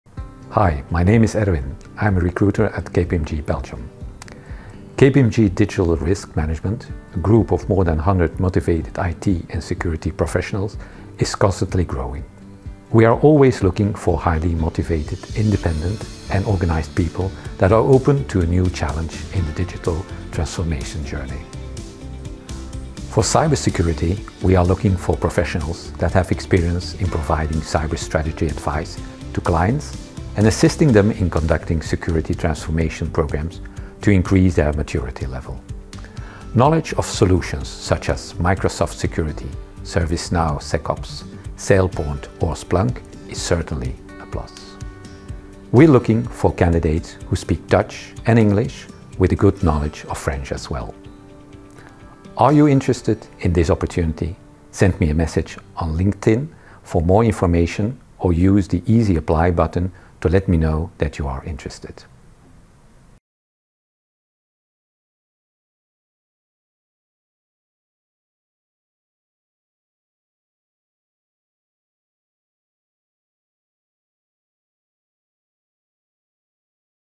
KPMG Careers - Digital Risk Management - Cyber Security
Our Digital Risk Management team is hiring! In this video you'll find out what a job in Cyber Security looks like at KPMG. Take a look at our vacancies and join us! shorturl.at/fkoz4